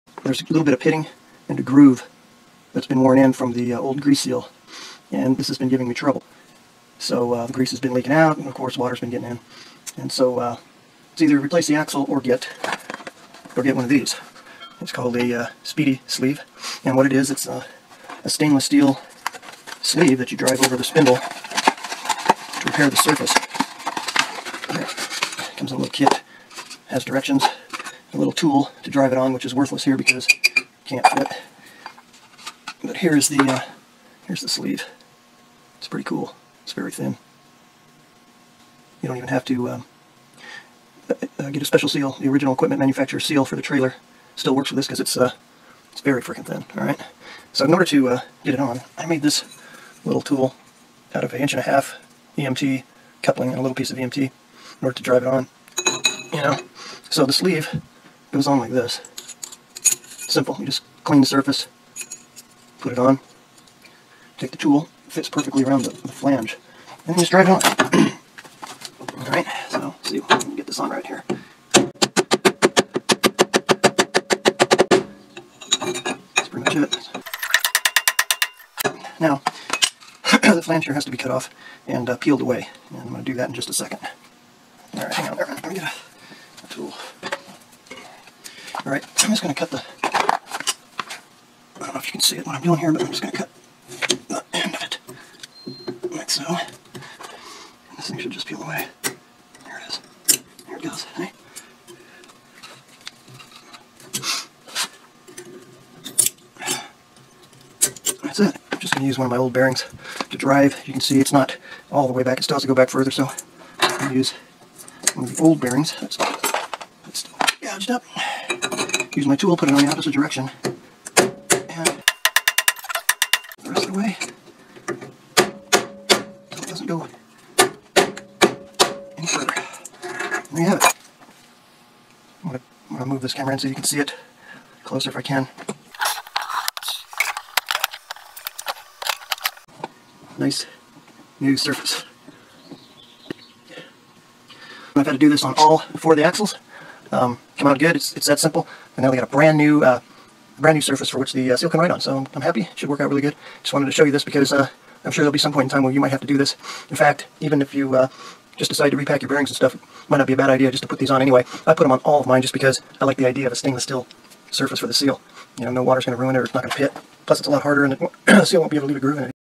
How to fix a trailer Stub Axles with pitting and a groove, instead of replacing it
there's a little bit of pitting
and a groove
that's been worn in from the old grease seal
and this has been giving me trouble
so the grease has been leaking out and of course
water's been getting in
and so
let's either replace the axle or get
or get one of these
it's called a
speedy sleeve
and what it is it's
a stainless steel
sleeve that you drive over the spindle
to repair the surface
there
comes a little kit
has directions
a little tool to drive it on
which is worthless here because
can't fit
but here is the
Here's the sleeve
it's pretty cool
it's very thin
you don't even have to
get a special seal
the original equipment manufacturer seal
for the trailer
still works with this because it's
it's very freaking thin
alright
so in order to get it on I made this
little tool
out of a inch and a half
EMT coupling and a little piece of EMT
in order to drive it on
you know
so the sleeve
goes on like this
simple you just clean the surface
put it on
take the tool fits perfectly around the flange
and then you just drive it on
all right
so see you get this on right here
it's pretty much it
now
the flangeshirt has to be cut off
and peeled away
and I'm going to do that in just a second
hang out there man
let me get
a tool
all right
I'm just gonna cut the
I don't know if you can see it what I'm doing here
but I'm just gonna cut
the end of it
so
this thing should just peel away
that's that
I'm just going to use one of my old bearings
to drive you can see it's not
all the way back it starts to go back further so
I'm going to use
one of the old bearings
gouged up
use my tool put it on the opposite direction
go away
doesn't go
further
I'm gonna move this camera and so you can see it
closer if I can
nice
new surface
I've had to do this on all
for the axles
come out good
it's that simple
and now they got a brand new
brand new surface for which the seal can write on
so I'm happy should work out really good
just wanted to show you this because
I'm sure there'll be some point in time when
you might have to do this
in fact even if you
just decide to repack your bearings and stuff
might not be a bad idea just to put these on anyway
I put them on all of mine just because
I like the idea of a stingless still
surface for the seal
you know no water's gonna ruin it or not gonna pit
plus it's a lot harder and
the seal won't be able to leave it grooving it either